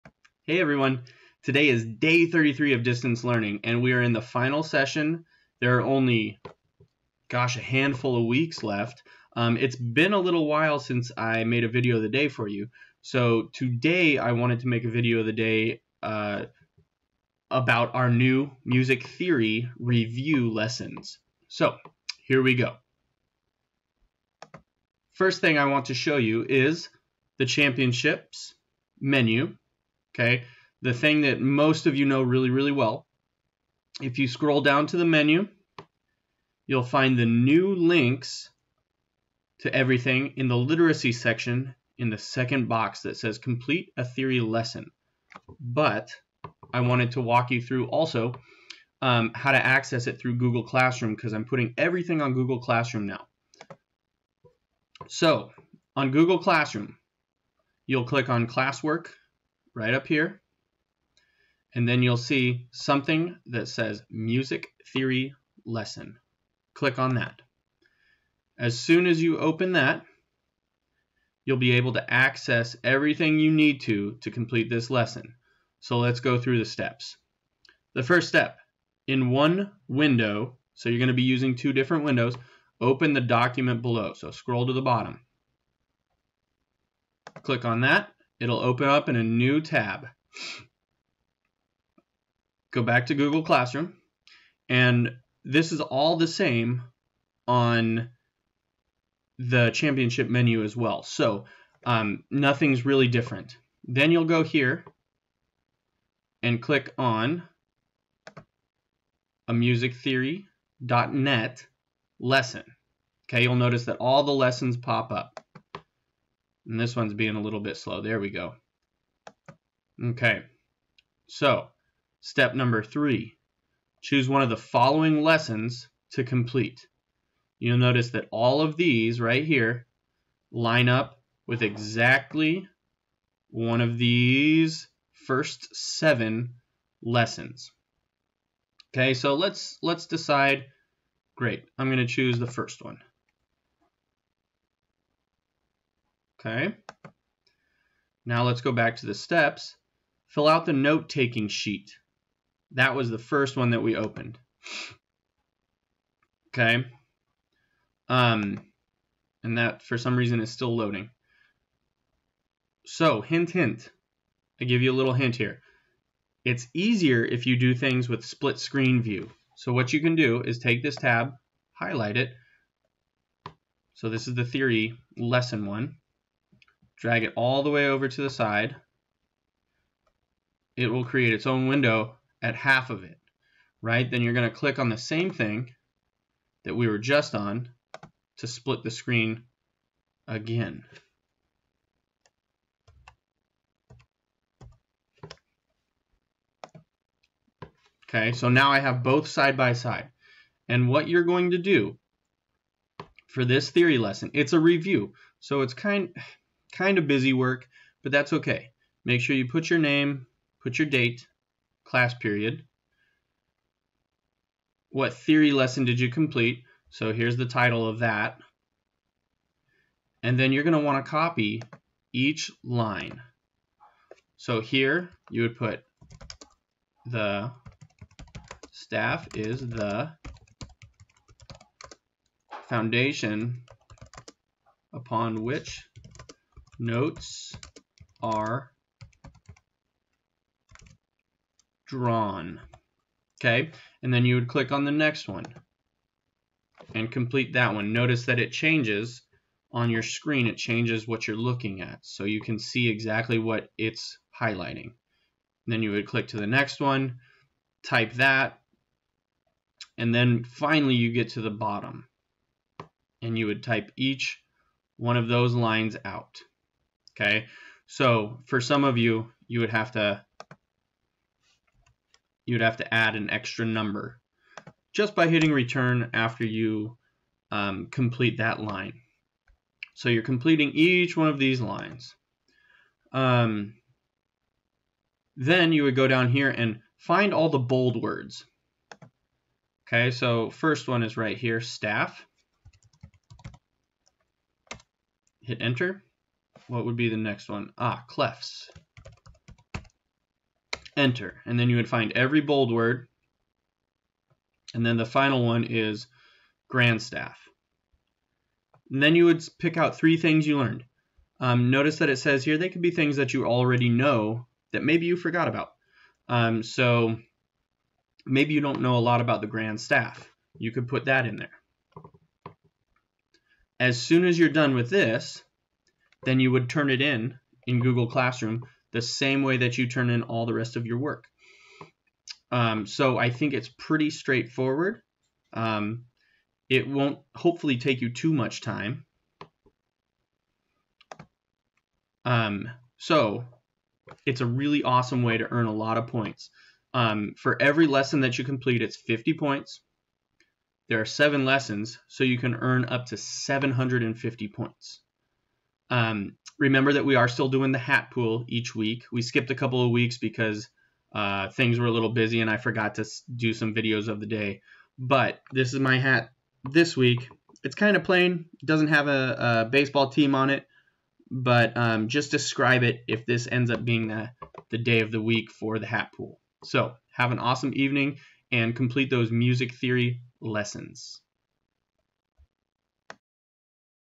Video of the Day - Day 33 - NEW Music Theory Lessons Tutorial
WHEW... It's been a while, but here's another Video of the Day.

This will explain how to complete the new Theory Lessons. It's a great review of the basic Music stuff we all should know.

Earn up to 350 Points if you complete all 7 lessons!!